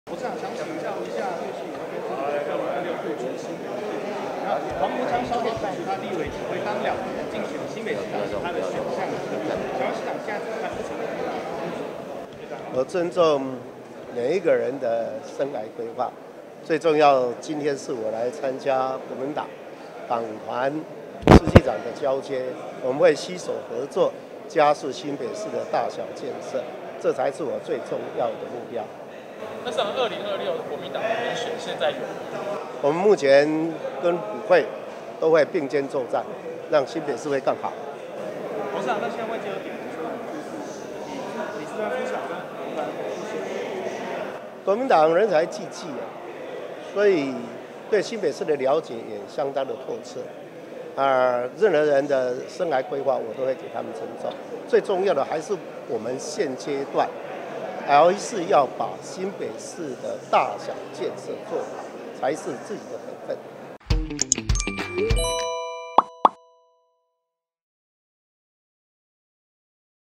黃國昌2026對戰李四川或劉和然？侯友宜：尊重每個人的生涯規劃【CNEWS】
CNEWS匯流新聞網記者陳儀潔、李文成、王聿瀠、陳巧紜、林佑威、施建宏／台北報導